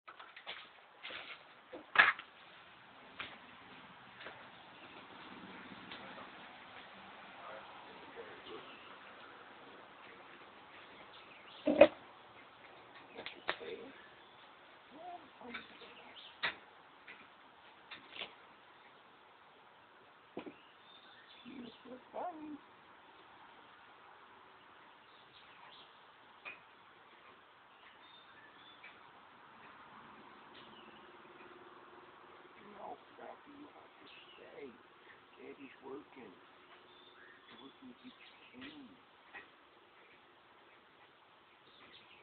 House rabbit getting groomed
My fiance' is grooming / brushing our pet Angora Lop house rabbit.